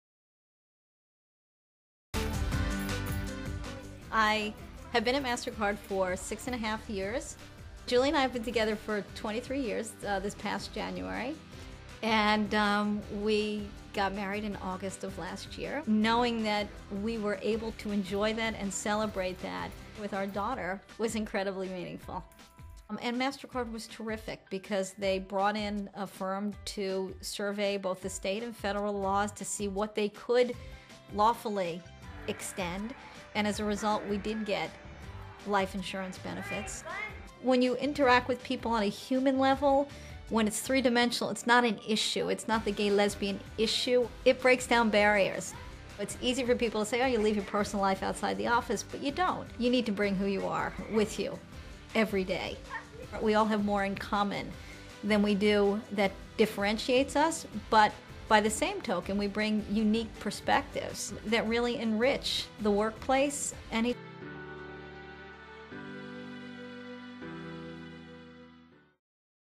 MasterCard's Abby Fiorella Reveals How Diversity Can Be Supported in the Workplace
Abby Fiorella, group head, global compliance and forensics for MasterCard, describes how her company supports her marriage to her same-sex life partner and their family. For more diversity and inclusion best practices, visit workingmother.com.